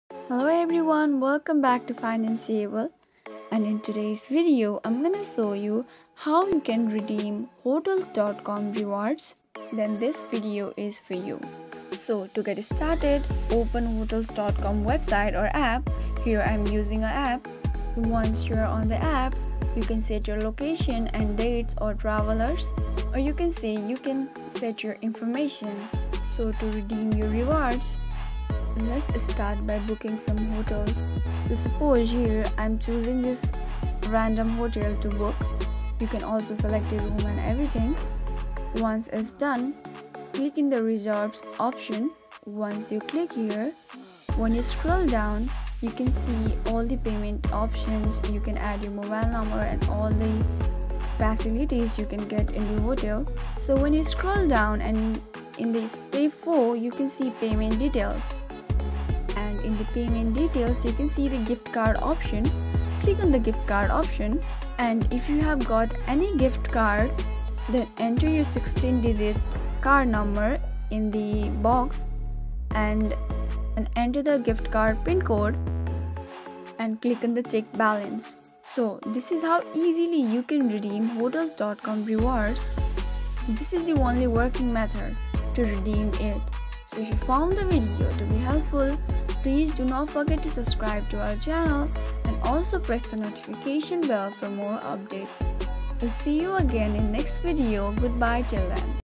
How to Reedem Hotels.com Rewards (2023)
Unlock the secrets of maximizing your Hotels.com rewards with our step-by-step tutorial! In this video, we guide you through the easy process of redeeming your hard-earned rewards on Hotels.com. Say goodbye to confusion and hello to free nights – watch now and make the most of your loyalty points!